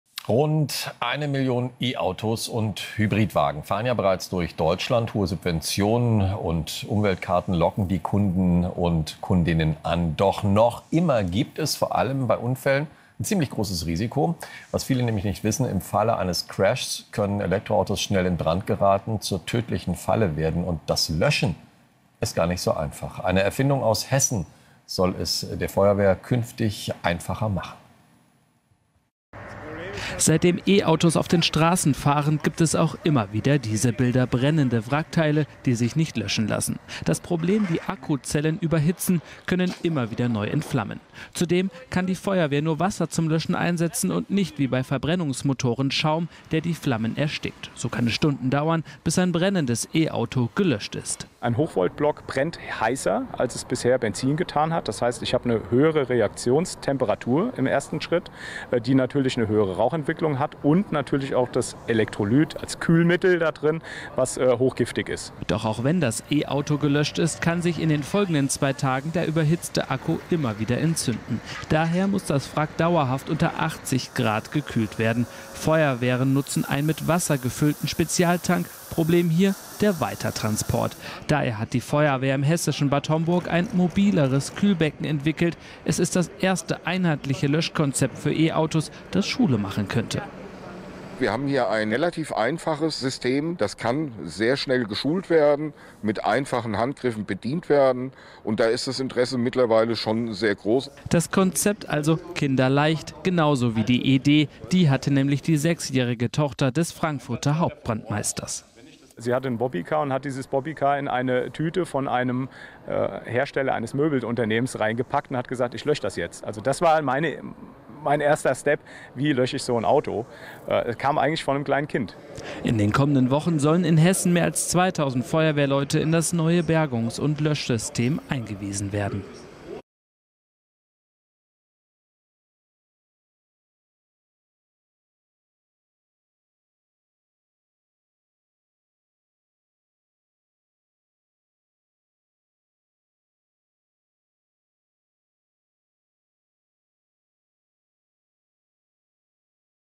E-AUTOS IN FLAMMEN: Was tun, wenn der Tesla brennt - Sechsjährige hat eine geniale Idee
Der E-Auto-Boom in Deutschland hat auch seine Schattenseiten, denn bei Unfällen geraten die Wagen schnell in Brand. Doch brennende Lithium-Ionen-Batterien stellen die Feuerwehr vor große Probleme. Diese Feuer lassen sich nicht so einfach löschen. Die Experten grübeln nun nach sicheren Lösch- und Lagerungsmethoden für überhitze Elektrofahrzeugen. Da hat die sechsjährige Tochter eines Frankfurter Hauptbrandmeisters eine geniale Idee.